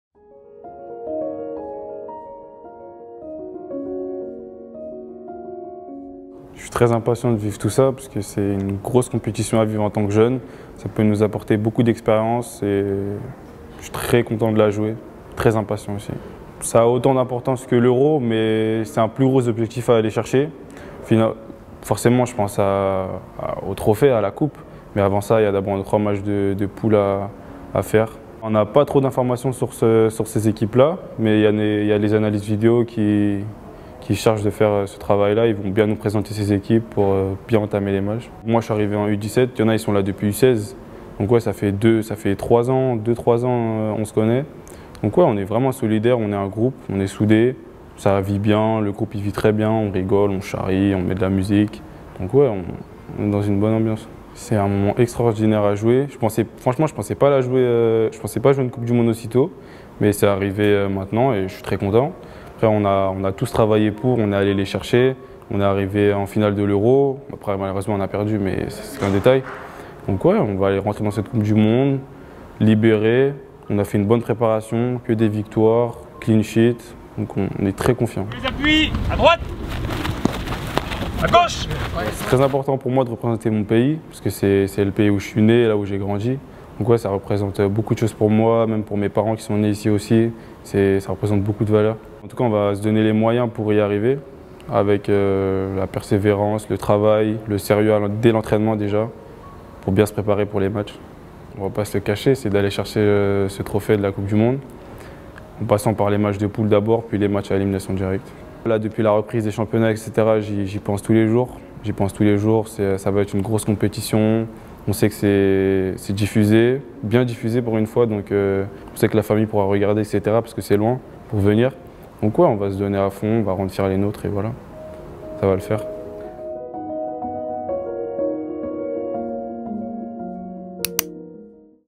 Mathis Lambourde : "Un moment extraordinaire"
A quelques jours du coup d'envoi de la phrase finale de la Coupe du Monde U17 de la FIFA en Indonésie, Mathis Lambourde (attaquant du Stade Rennais) nous partage son enthousiasme et les ambitions du groupe France.